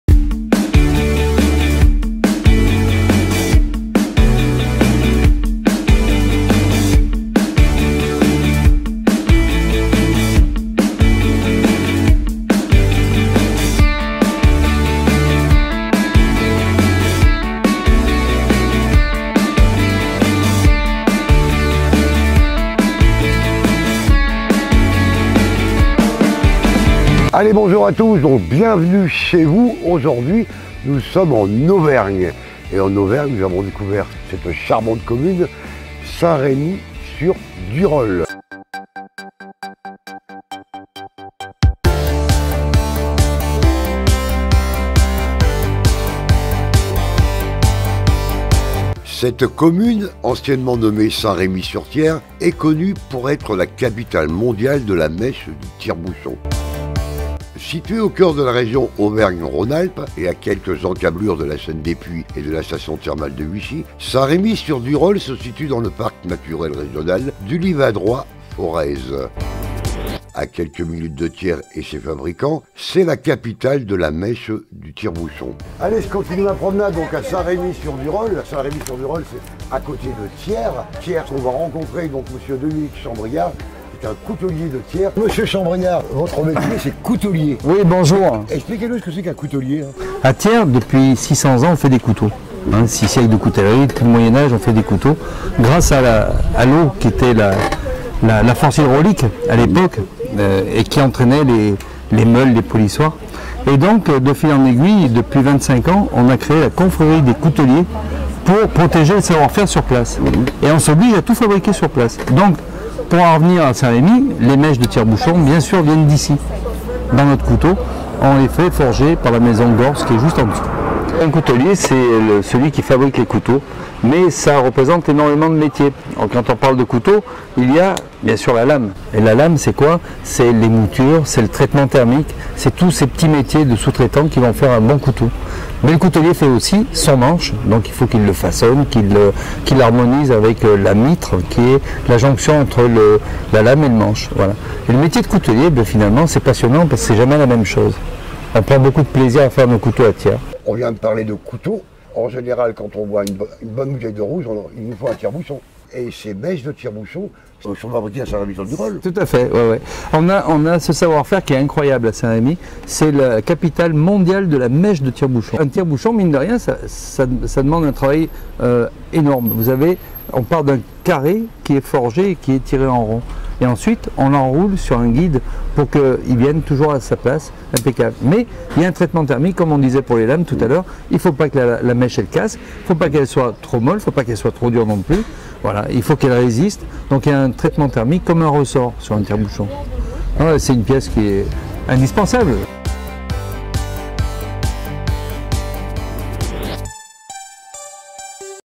Bienvenue chez vous.Indissociable de la mèche du tire bouchon, le couteau.
Bienvenue chez vous.
Indissociable de la mèche du tire bouchon, le couteau. Thiers, la capitale française de la coutellerie, la coutellerie Chambriard

ICA INFO TV
site Replay :  icainfotv.fr